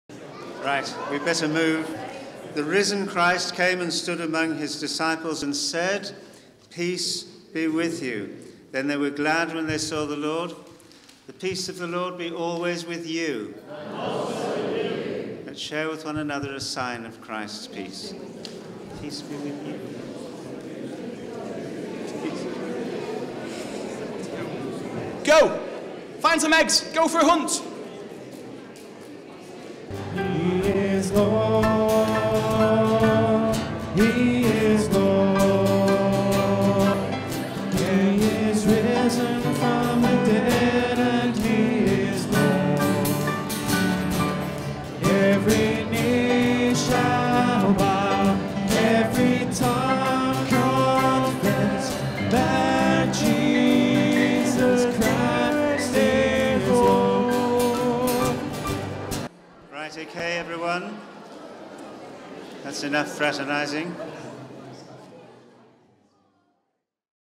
Sharing the Peace on Easter Sunday
The first time in over 2 years we've shared the peace 'properly'